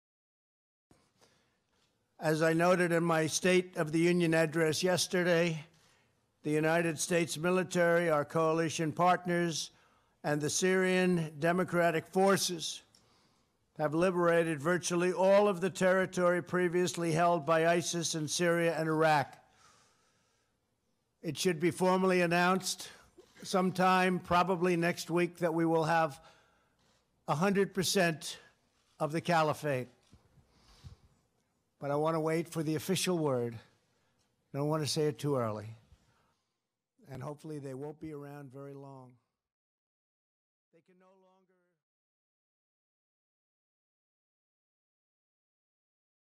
DONALD TRUMP adelanta el fin del Estado Islámico
El presidente de EE UU asegura que el califato ha sido diezmado y confiesa que "nadie pensó que fuera posible hacerlo tan rápido".